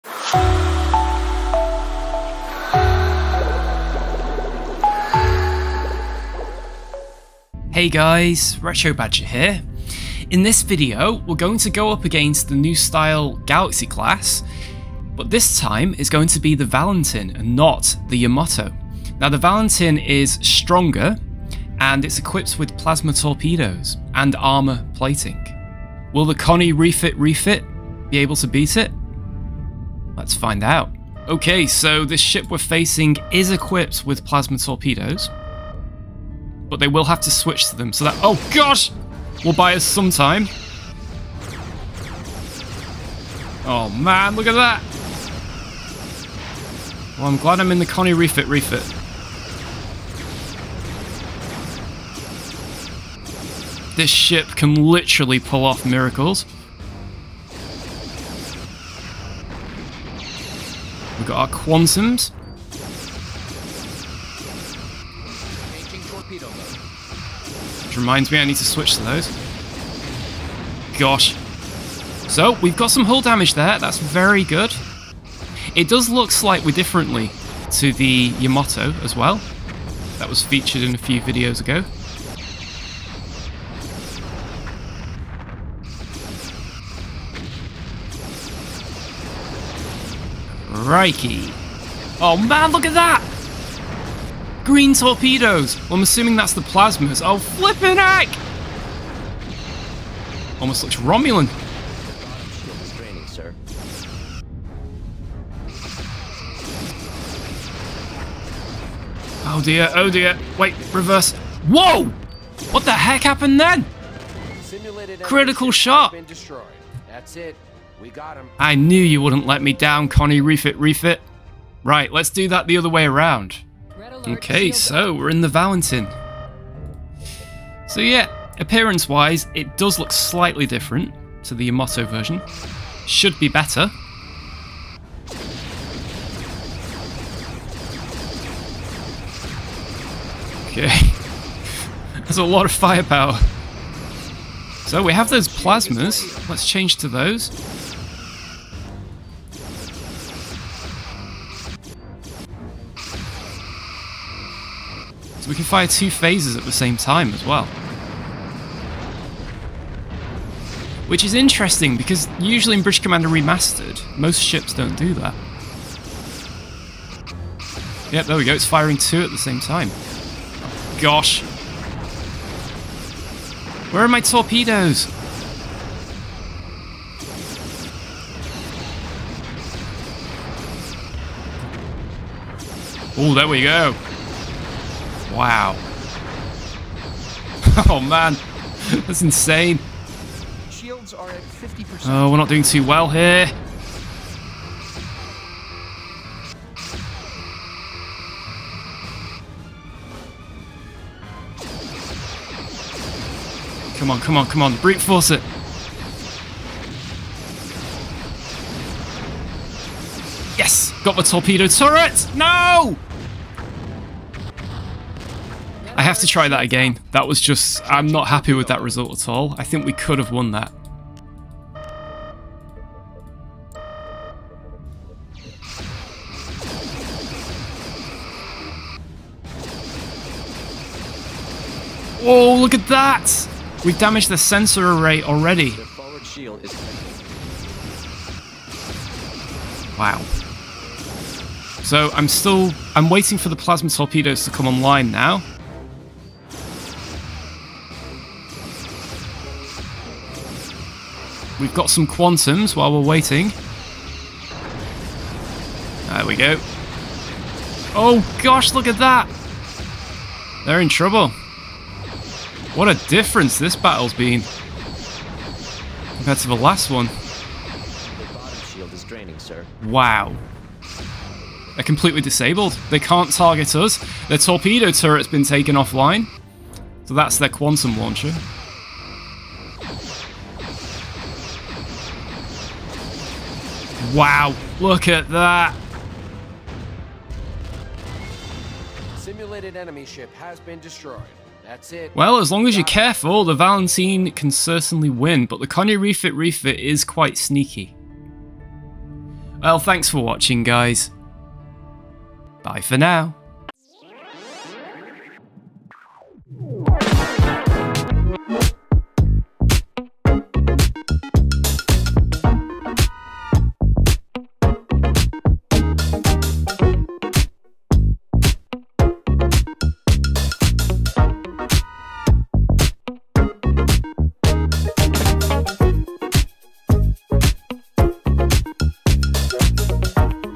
NEW Galaxy Class Valentine VS Connie Refit Refit - BOTH SIDES - Star Trek Ship Battles -
In this video we put the New Galaxy Class Variant the Valentine against the Connie Refit Refit. It's equipped with Plasma Torpedoes that look suspicioulsy Romulan!